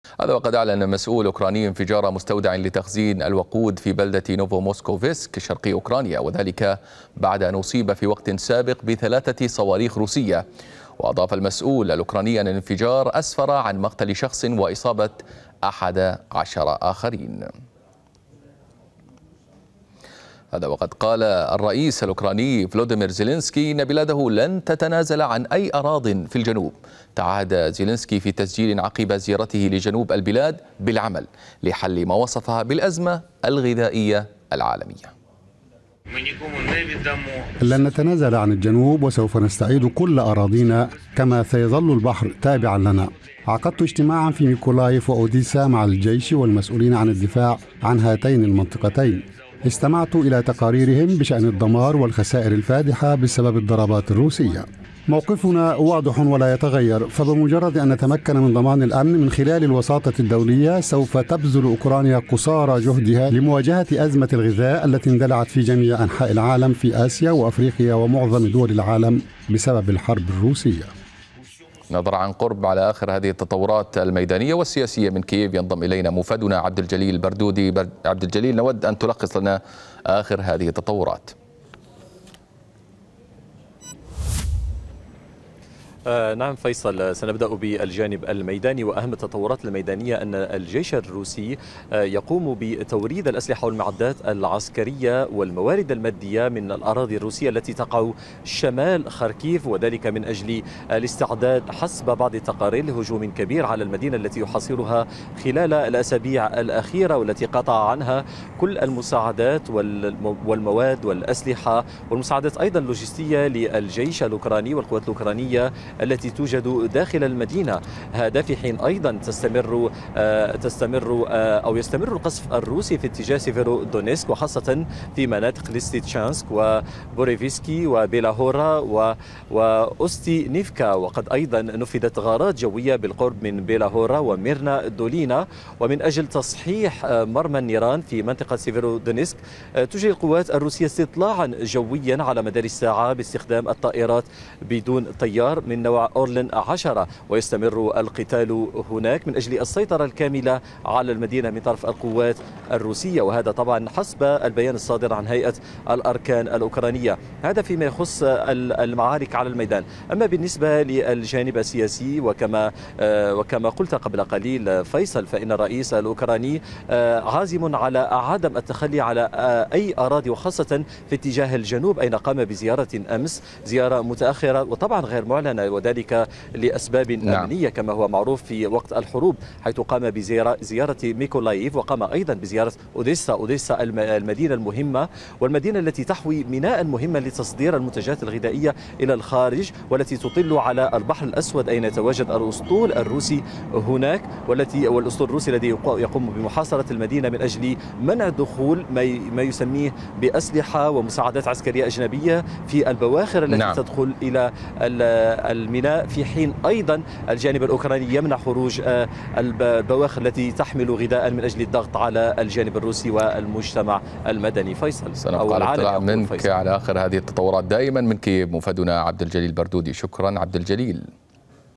باستخدام صواريخ مجنحة.. روسيا تدمر مستودعات للوقود في أوكرانيا
أعلن مسؤولٌ أوكراني انفجارَ مستودع لتخزين الوَقود في بلدة نوفوموسكوفيسك شرقيَّ أوكرانيا،
 وذلك بعد أن أصيب في وقت سابق بثلاثةِ صواريخَ روسية.
وأضاف المسؤول الأوكراني أن الانفجار أسفر عن مقتل شخص وإصابةِ أحدَ عشر آخرين.